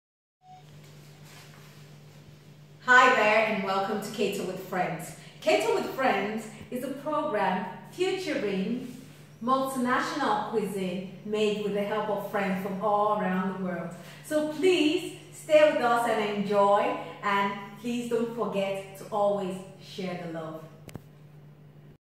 Welcome guys!!
Hi there!!

Welcome to “Keto With Friends”. We are going to take you on a journey of a lifetime. A ride where we will soak up cultural diversities whilst enjoying and learning how to make low carb, delicious dishes from different continents across the globe with help of my multinational team of best friends. Moreover, this is going to be fun!!

Where we live is a fantastic community comprising of beautiful personalities from round the World. We have had the privilege to experience an explosive fusion of home cooked meals from savory desserts to sweet meals. The first time I tasted cauliflower rice I couldn't believe it wasn't rice and I honestly preferred it.

That was when my journey began. I started enjoying low carb meals, then a friend told me about keto. since then, I have mastered experimenting with different meals, sharing recipes with friends and with the help of my friends we are happy to share with you.

We will help make your lifestyle truly delicious, with picture tutorials and videos of various meals, desserts and drinks. Lets enjoy the journey together.

follow our facebook page @ketowithfriends